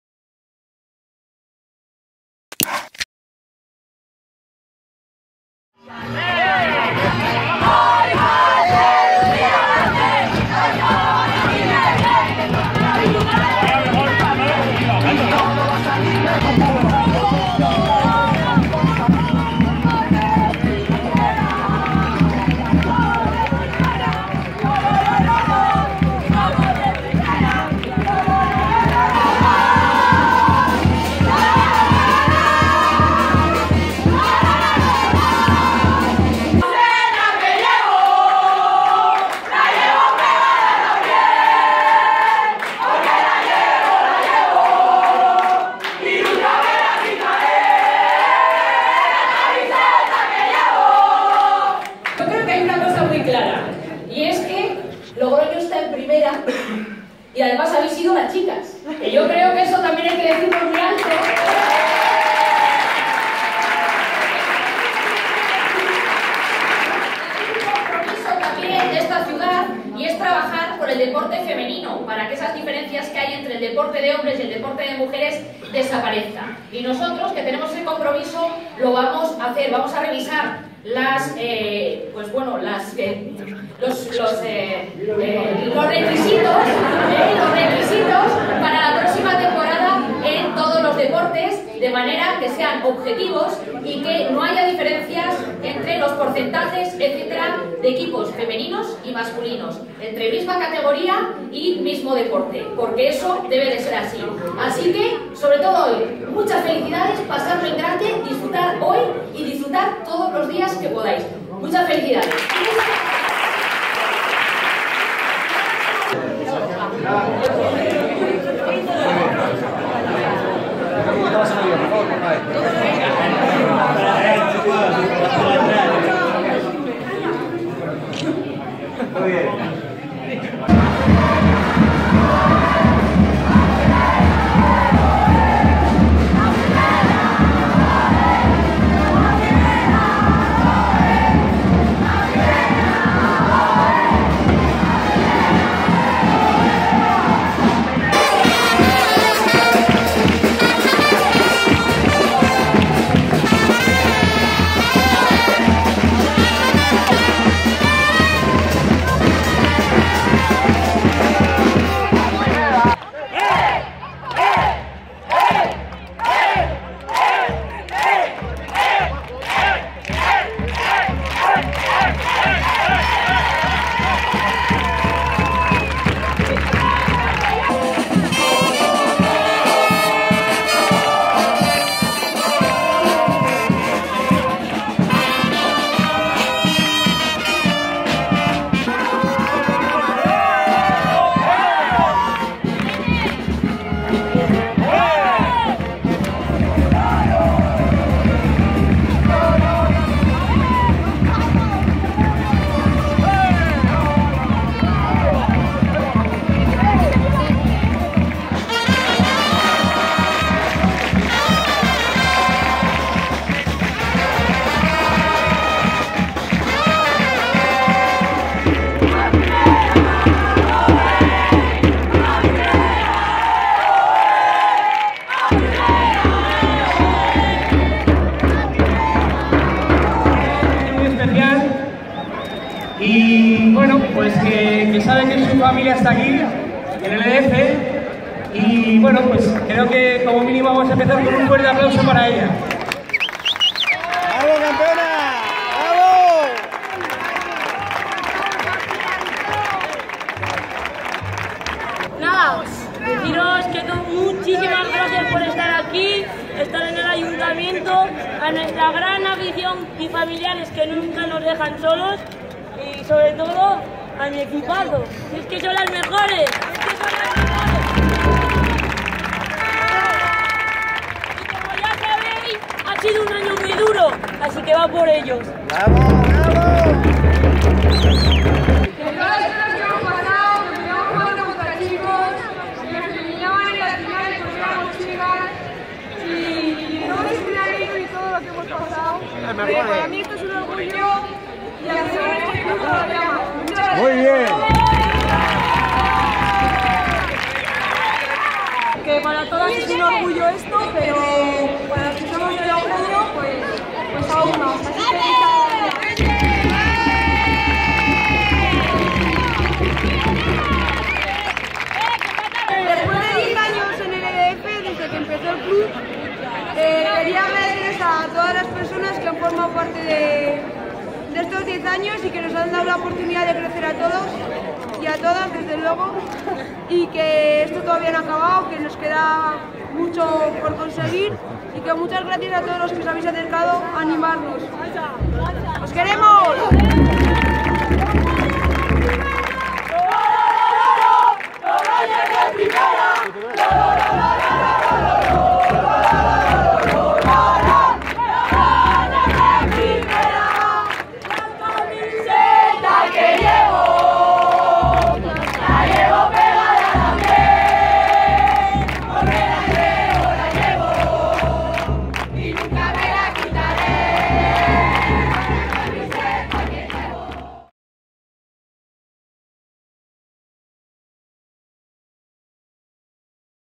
Celebración del EDF por su ascenso a Primera División
¡Logroño es de Primera! Así han celebrado las chicas del EDF su ascenso a la élite del fútbol nacional.